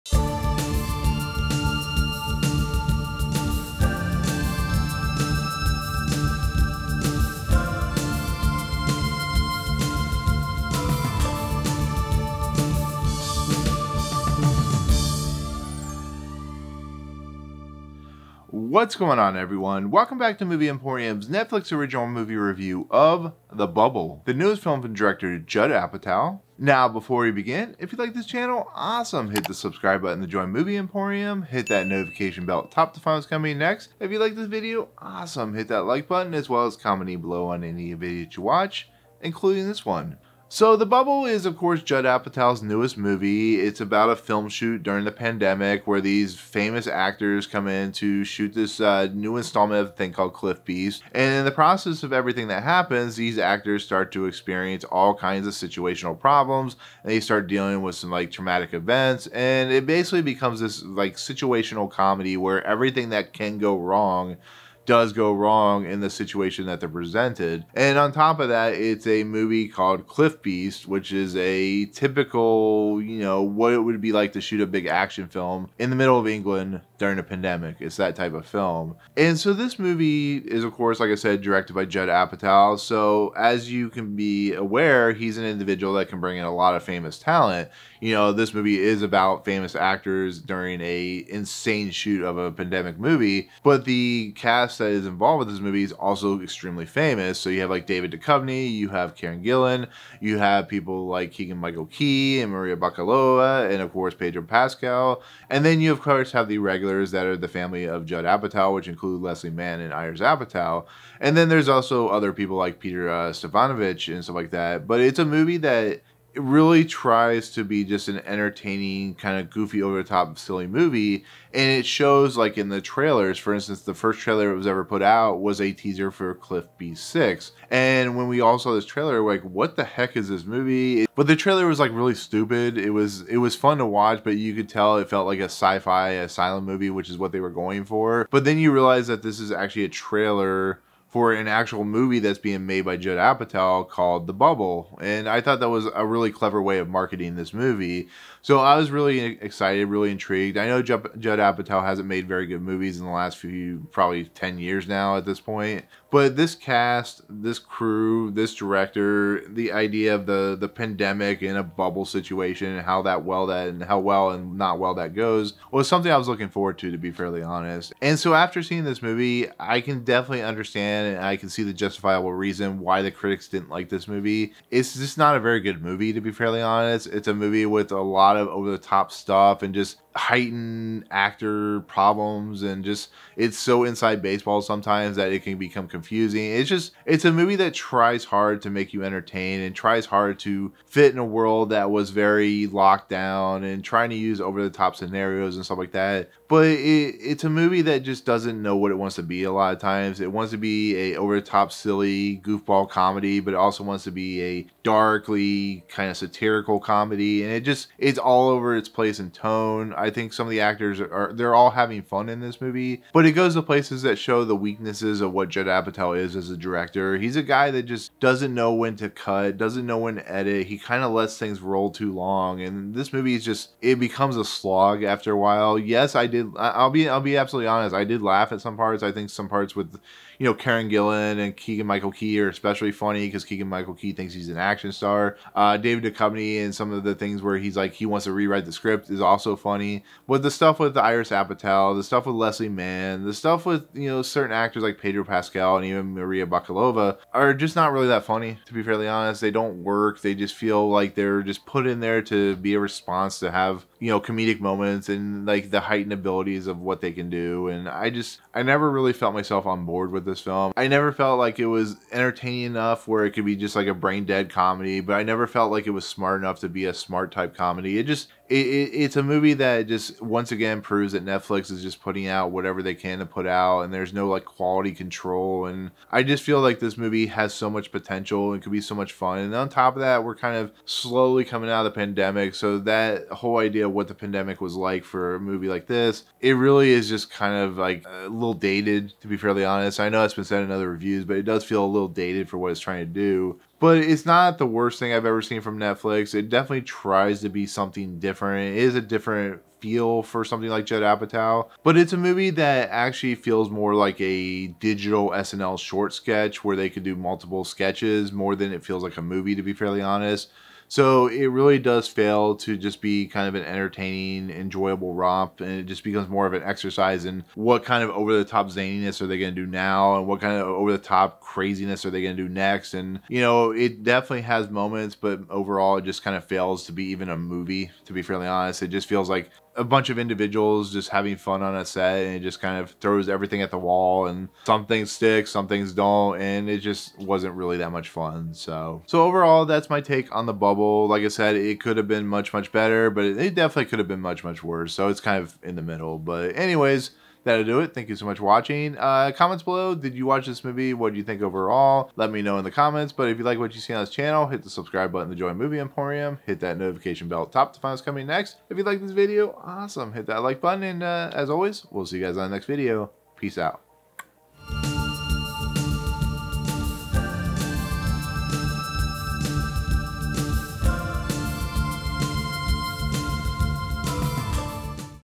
The Bubble - Netflix Reviews
A group of actors and actresses begin to film the latest Hollywood blockbuster during the pandemic, but things start to spiral out of control when filming turns into a disaster.

The Bubble is Directed by: Judd Apatow and Stars: Karen Gilla, Iris Apatow, Pedro Pascal, Leslie Mann, David Duchovny, Keegan-Michael Key, Fred Armisen, Guz Khan, Harry Trevaldwyn, Maria Bakalova, Kate Mckinnon, Chris Witaske, Dave SImon, Danielle Vitalis with Peter Serafinowicz and Maria Bamford.

0:00 Intro
0:40 About The Bubble
02:47 Review
05:46 Outro

Music orchestrated by Dan Jensen, you can find his content and find him on twitter @danjensenmovies